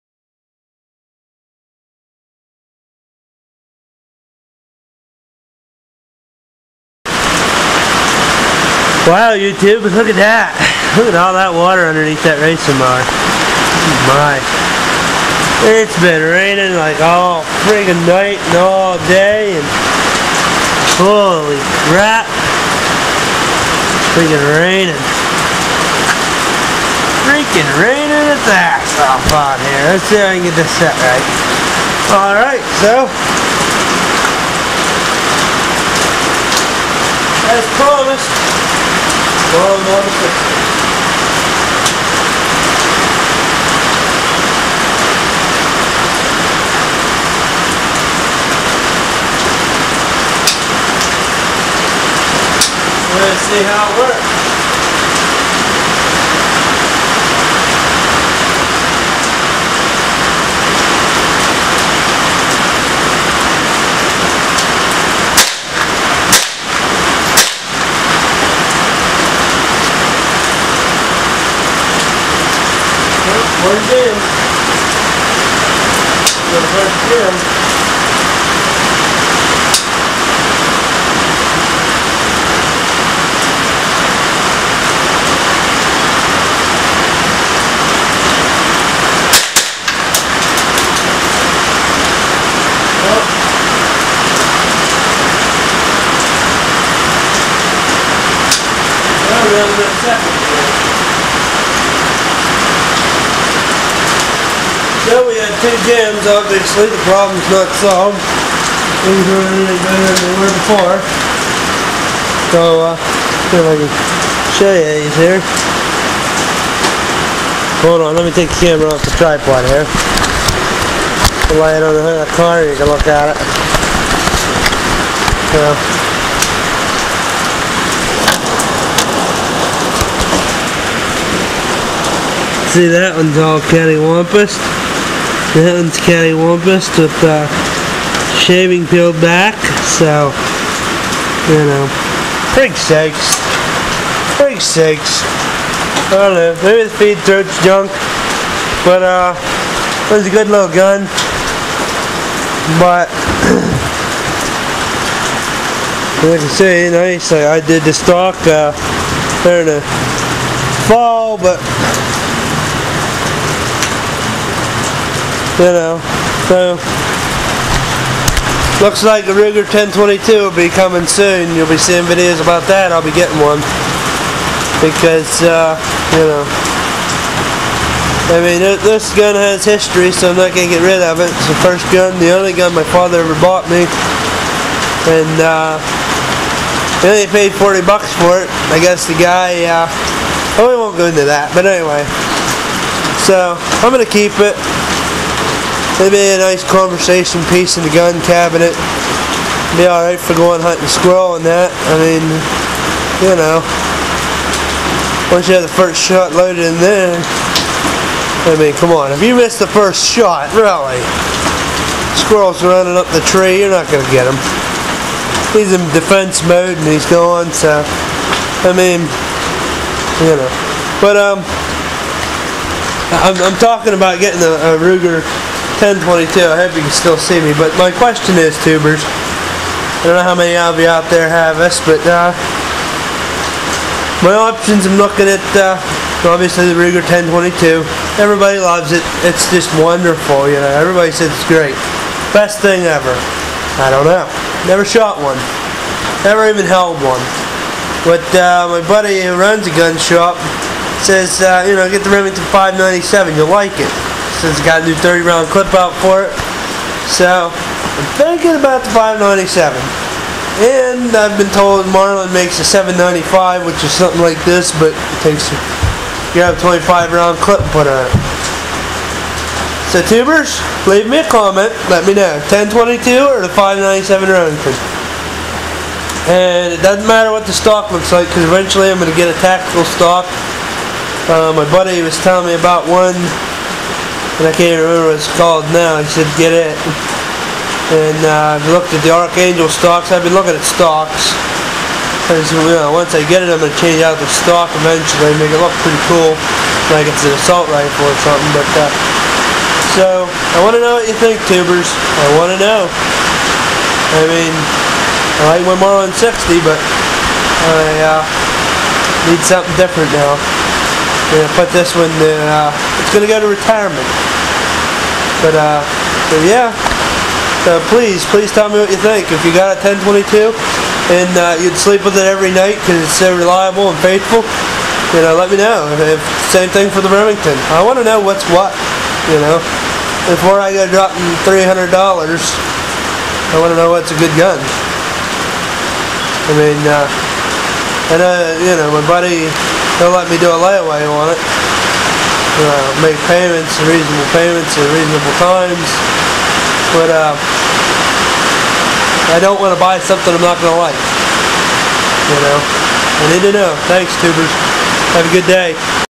marlin model 60
well i took it out and it jammed twice frigg sakes wtf so i guess im gonna buy a remington 597 someday soon i dont know